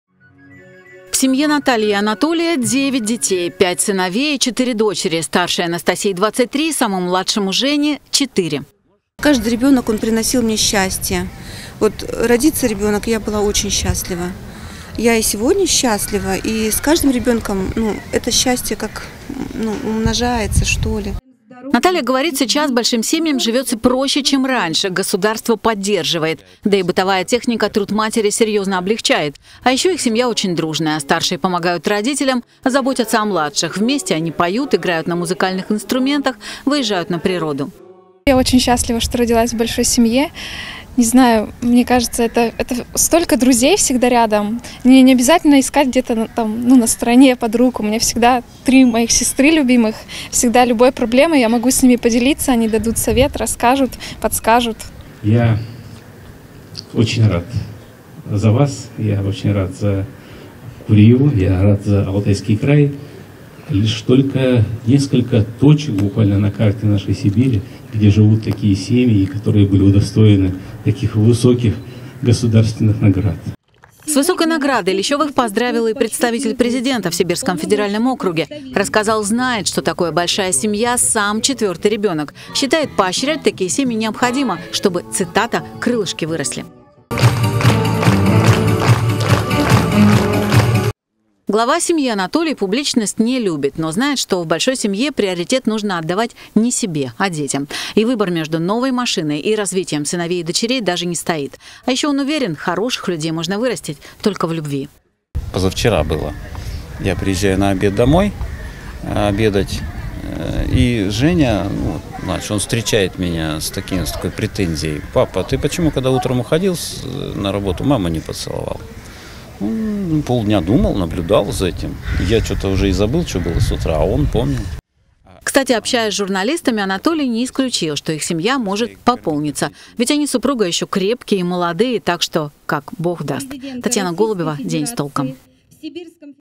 Многодетная семья Лещёвых из Курьи получила орден «Родительская слава»
Многодетная семья Лещёвых из Курьи получила орден «Родительская слава». Высокую государственную  награду  родителям девяти детей вручили накануне. К слову, достойны ее лишь те, в чьей семье более семи  ребятишек и кто по – настоящему воспитывает их - заботится о здоровье, образовании, духовном и нравственном развитии